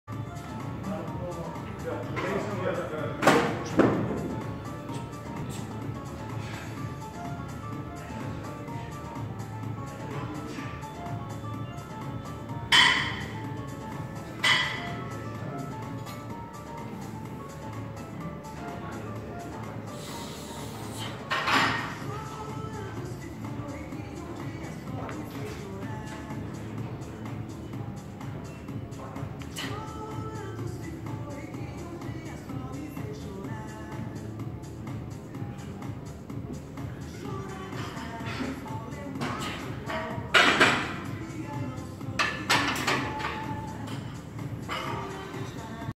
Sanne 110 kg x 3 squat paused weightlifting style
Hoi! Na mijn overwinning op de DRC-Cup 11 juni ben ik mij nu aan het voorbereiden voor de SBD-Cup op 24/25 september te Nijmegen. Met SBD als mijn sponsor en Joost als mijn coach gaat dit helemaal goedkomen!

-Classic powerlifter -84 kg - Instagram: Sanne_DutchPower